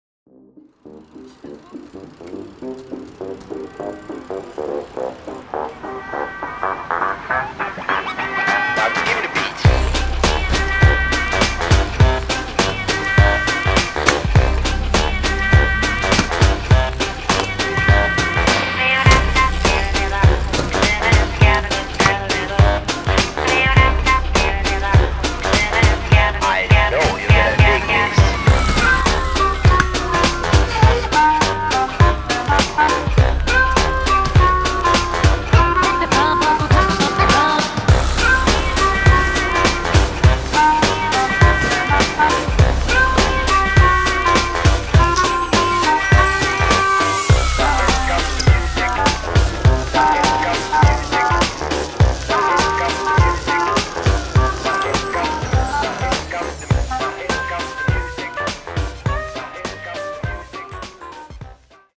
Stay Healthy!
These Clips were filmed between breaking my wrist in spring and falling on it again some months ago. Being injured sucks and its even a worse thing when its seems as your body doesnt even try to heal up. So my advice for all of you: Stay healthy!